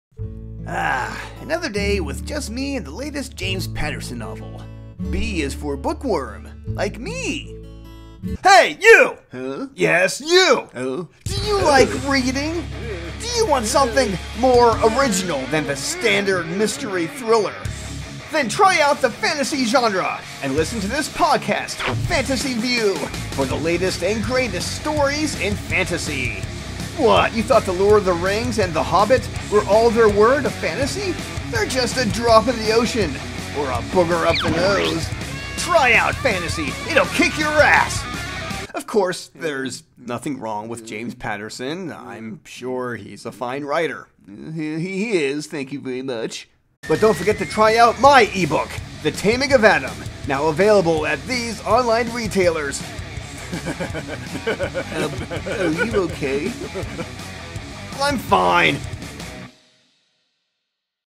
Fantasy 'View intro
Welcome to Fantasy 'View, where I take a look at the latest and greatest fantasy novels!